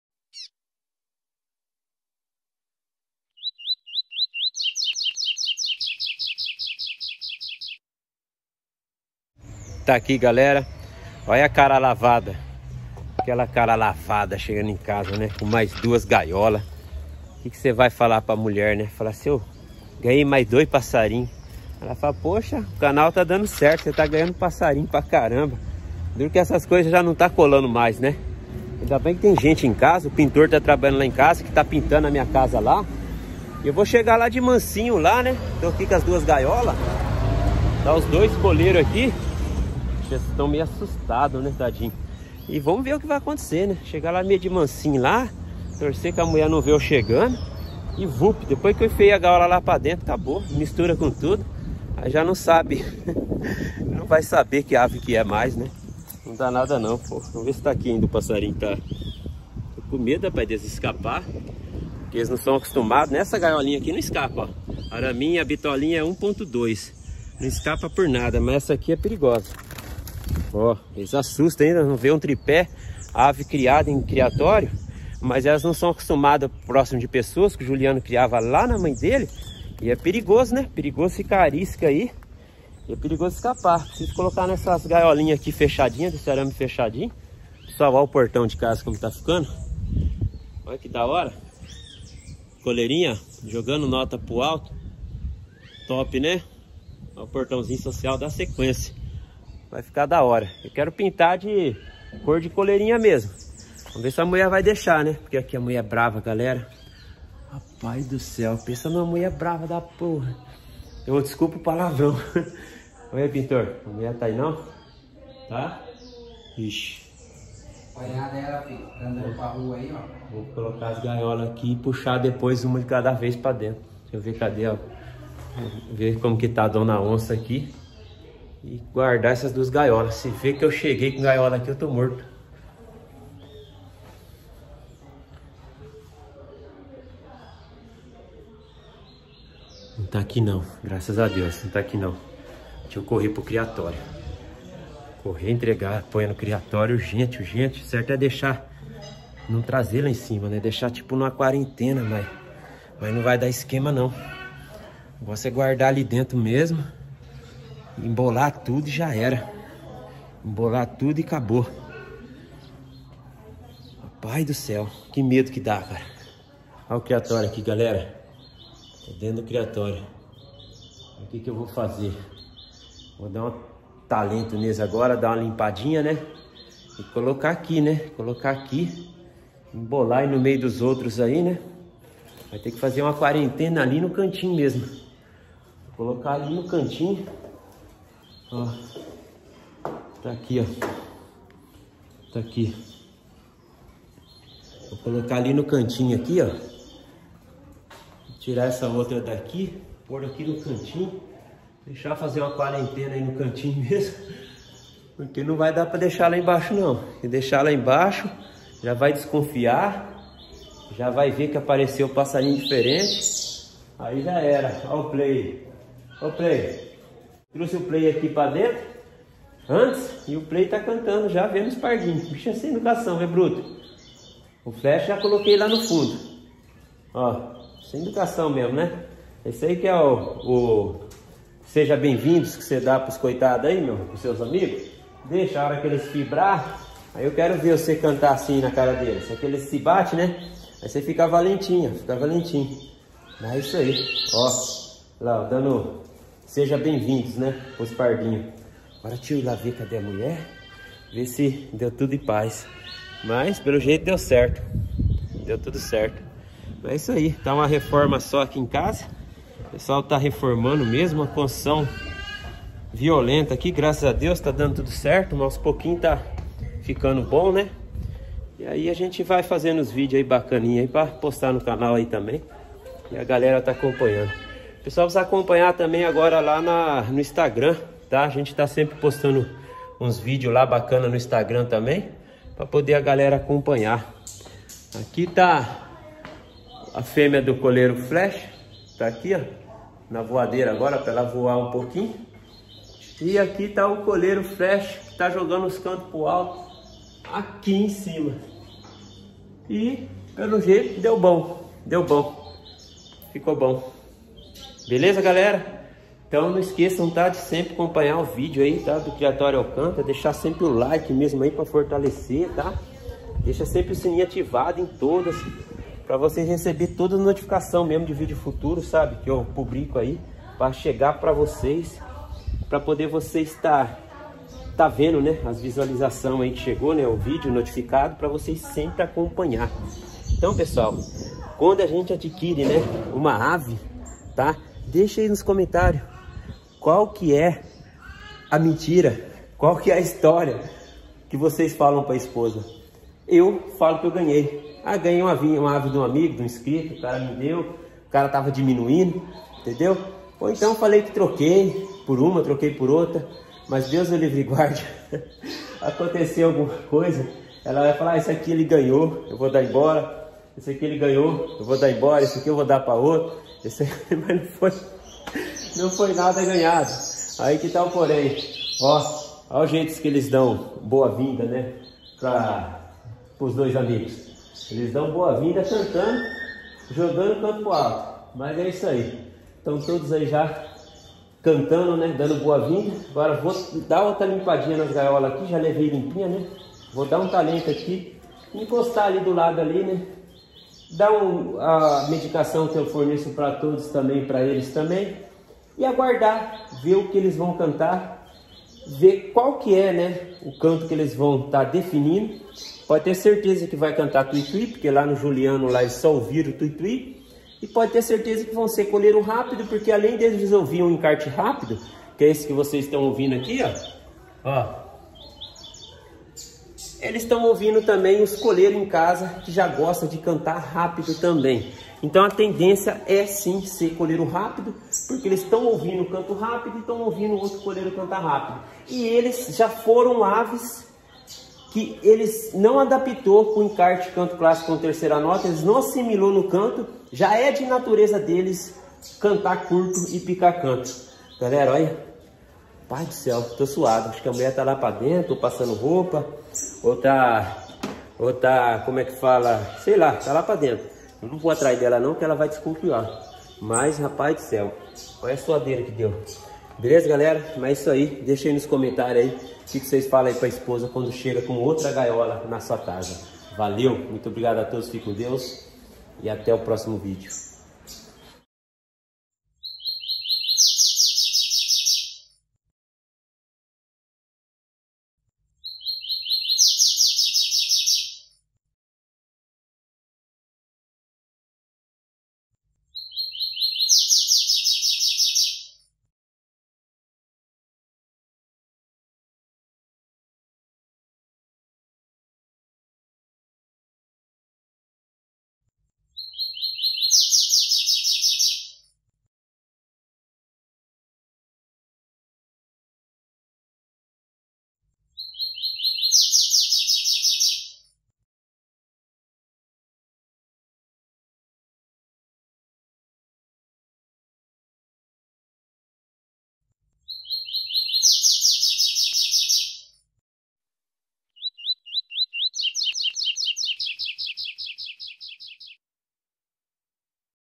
CHEGANDO COM 2 COLEIRO EM CASA (escondido da esposa)
Neste vídeo passei o maior sufoco chegando em casa escondido com 2 novos coleiros para o criatório. Fui bem de mansinho para que minha esposa não veja. Qual a história que você conta para a dona onça quando chega com uma nova ave? DEIXE NOS COMENTÁRIOS QUE EU VOU CURTIR.
CAIXA POSTAL - 1009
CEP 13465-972
Americana SP

CONTATOS:
APOIADOR do Canal:
SELLECTA ROVANI: O Alimento Do Seu Pássaro.